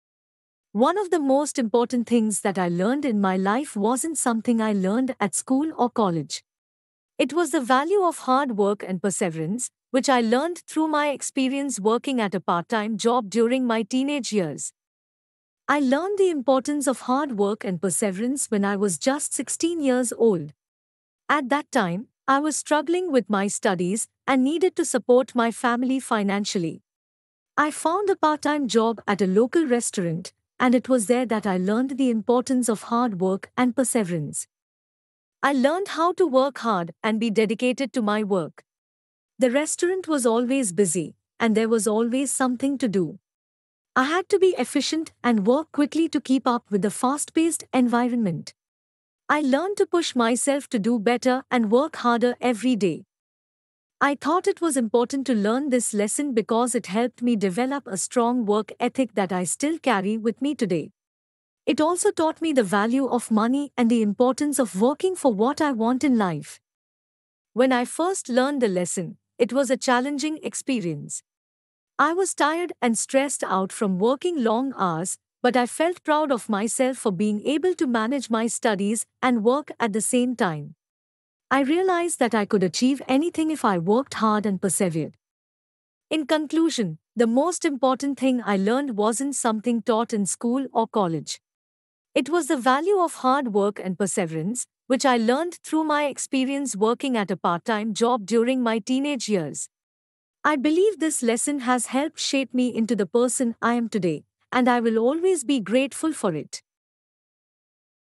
Describe an important thing you learned, not at school or college.
- When did you learn it?
- How did you learn it?
- Why did you think it was important to learn it?
- How you felt when you learned it?